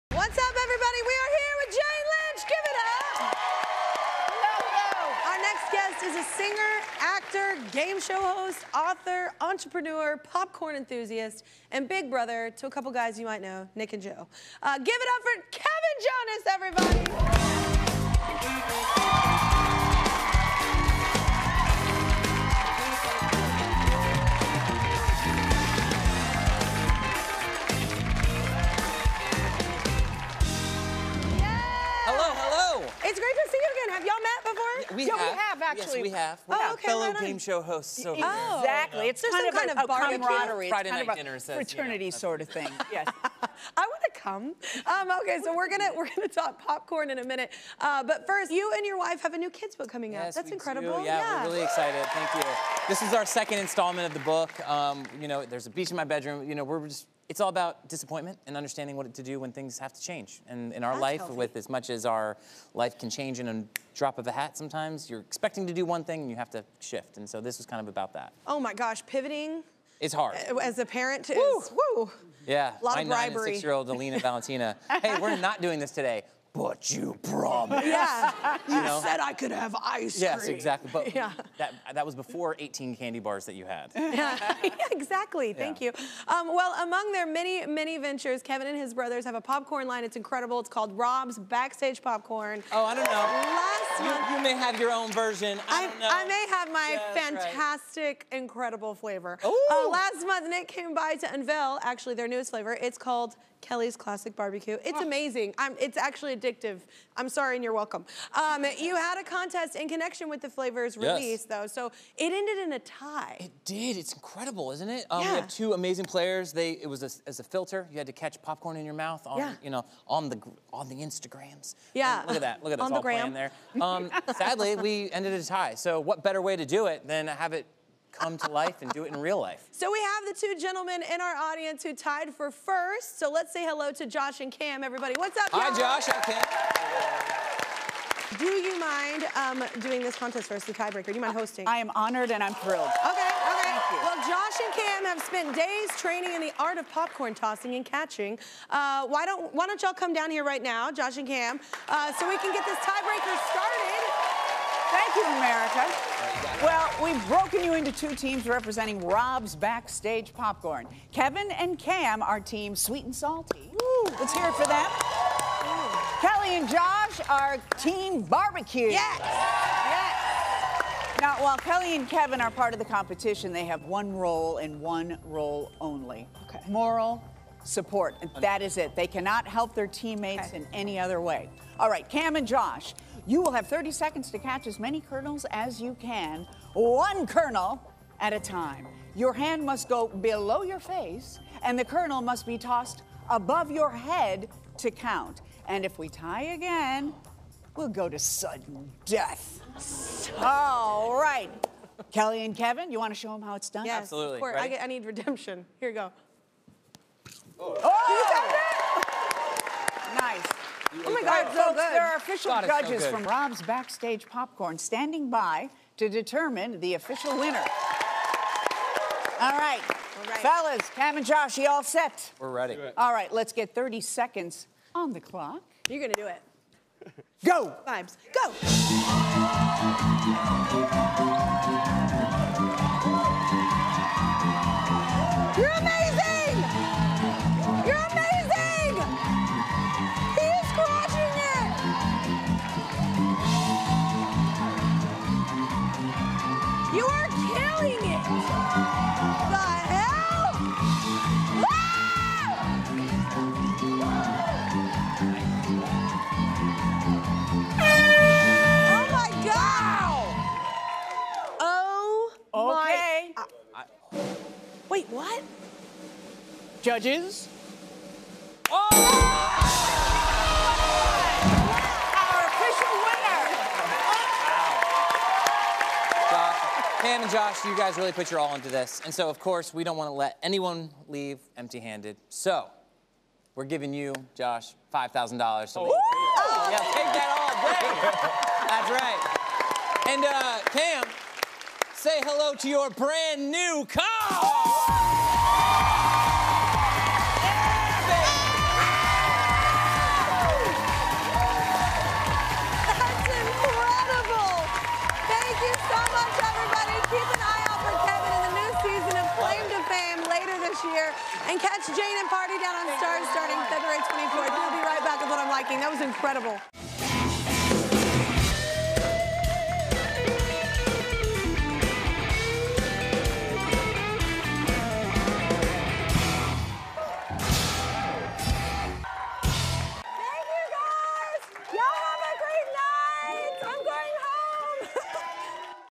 Kevin Jonas & Kelly Clarkson Crown Winner Of Rob's Backstage Popcorn Challenge
The Rob's Backstage Popcorn Kernel Catching Contest ended in a tie on Instagram, so Kevin Jonas and finalists Josh and Cam are here to settle the score IRL! Watch Josh and Cam race against the clock to see who can catch the most kernels in 30 seconds — with a brand new car on the line!